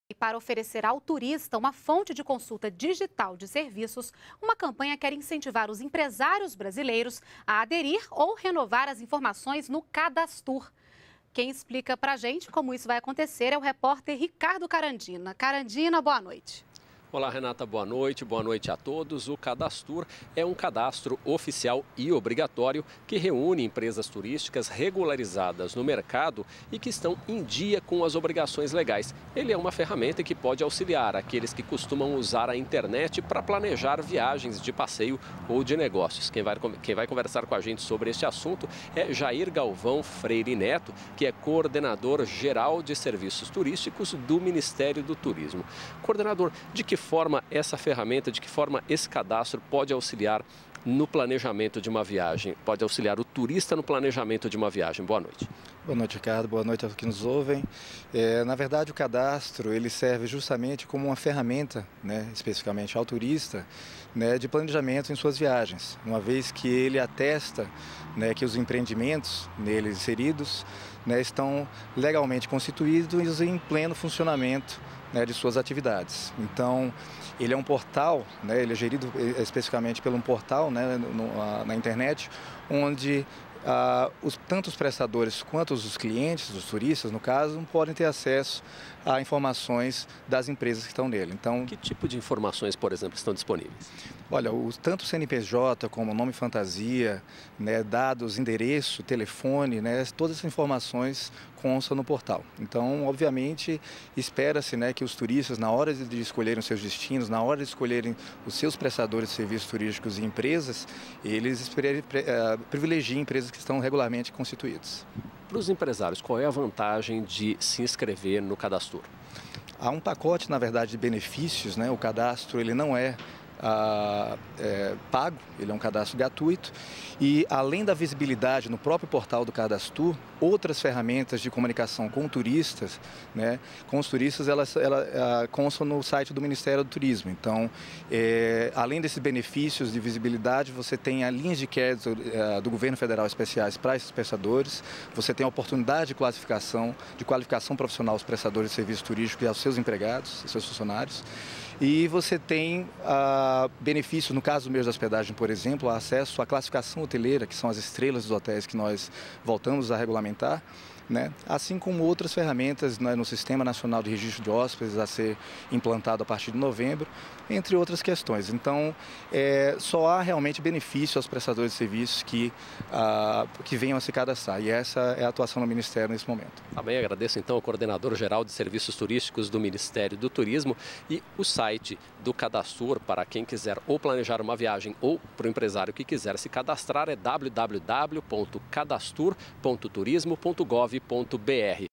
Campanha busca aumentar a adesão das empresas turísticas no portal CadasTur
NBR NOTÍCIAS - 10.10.12: Pessoas que pretendem viajar de férias ou a negócio podem acessar o portal do Cadastro do Turismo (CadasTur) para encontrar informações sobre a regularidade da atividade ou establecimento escolhidos. Para aumentar o banco de dados, o Ministério do Turismo realiza campanha de incentivo para ampliar o número de empresas brasileiras inseridas no sistema. O coordenador de Qualificação e Serviços Turísticos, Jair Neto, explica que a ferramenta atesta o cumprimento das obrigações legais do estabelecimento.